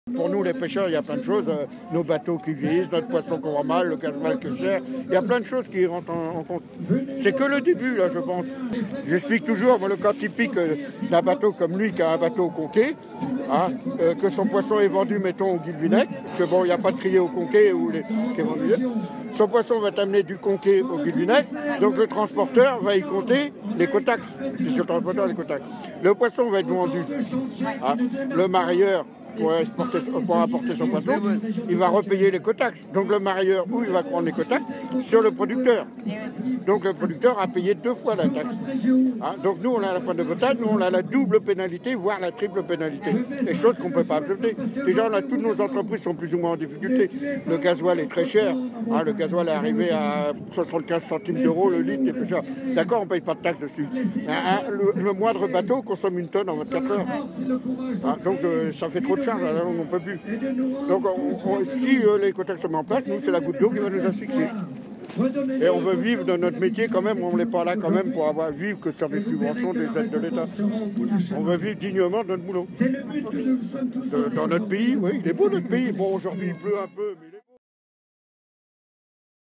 Ecotaxe : Le témoignage du pêcheur Philippe Duval
Notre reporter Pauline Lallement a recueilli le témoignage de Philippe Duval, pêcheur au Guilvinec en Bretagne, lors de la manifestation contre l'écotaxe samedi 2 novembre à Quimper.